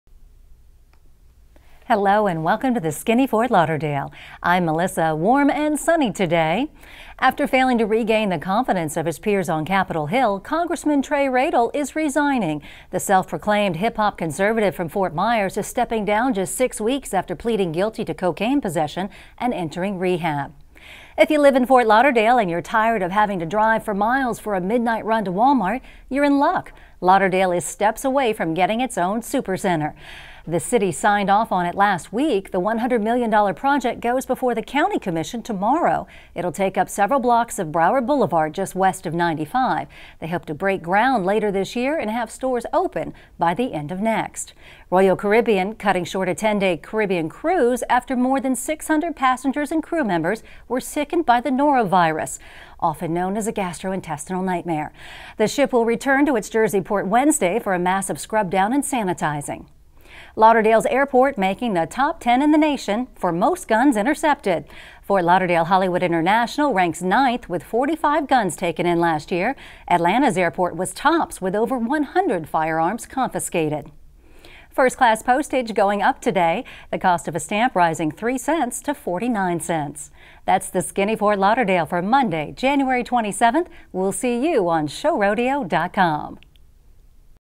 45 Guns Don't Catch Their Flight at Fort Lauderdale Airport
Hello and welcome to the Skinny Fort Lauderdale.

I'm Melissa. Warm and sunny today.

After failing to regain the confidence of his

peers on Capitol Hill, Congressman Trey Radel

is resigning.  The self-proclaimed hip-hop

conservative from Fort Myers is steppng down

just 6 weeks after pleading guilty to cocaine

possession and entering rehab.

If you live in Ft. Lauderdale and you're tired

of having to drive for miles for a midnight run

to Wal-mart, you're in luck.  Lauderdale is steps

away from getting it's own Super Center!  The city

signed off on it last week. The 100-million dollar

project goes before the county commission tomorrow.

It'll take up several blocks of Broward Boulevard

just west of 95. They hope to break ground later

this year and have stores open by the end of next.

Royal Caribbean cutting short a 10-day Caribbean

cruise, after more than 600 passengers and crew

members were sickened by the Norovirus, which is

known as a gastrointestinal nightmare.  The ship

will return to its Jersey port Wednesday, for a

massive scrub down and sanitizing.

Lauderdale's airport making the top ten in the nation

for the most guns intercepted! Ft. Lauderdale Hollywood

International Airport ranks 9th with 45 guns taken in

last year. Atlanta's airport was tops with over 100

firearms confiscated.

First class postage going up today: The cost of a

stamp rising 3 cents to 49-cents.

That's the Skinny Fort Lauderdale for Monday,

January 27th. We'll see you on Showrodeo.com.